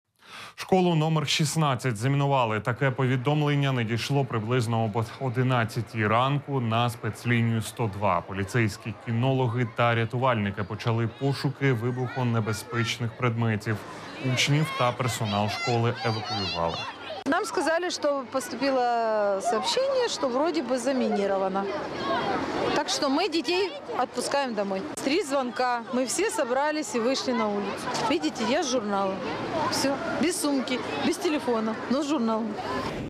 Невідомі повідомили про вибухівку в школі
Школу № 16 замінували — таке повідомлення надійшло приблизно об 11-й ранку на спецлінію «102».

Поліцейські, кінологи та рятувальники почали пошуки вибухонебезпечних предметів. Учнів та персонал школи евакуювали.

«Нам сказали, что поступило сообщение, что вроде заминировано. Так что мы детей отпускаем домой. Три звонка, мы все собрались и вышли. Вот видите, я с журналом — без сумки, без телефона, но с журналом», — розповіла вчитель початкових класів ЗОШ № 16 Людмила Демченко.